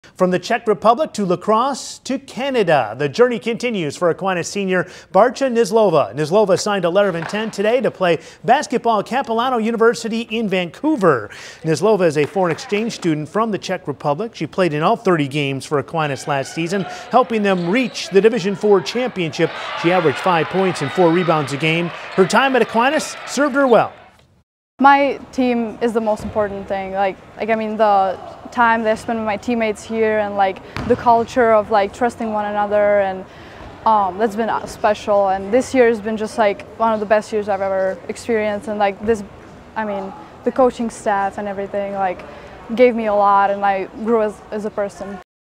Hnizdilova will continue hoops career in Canada
From the Czech Republic to La Crosse to Canada. The journey continues for Aquinas senior Barcha Hnizdilova.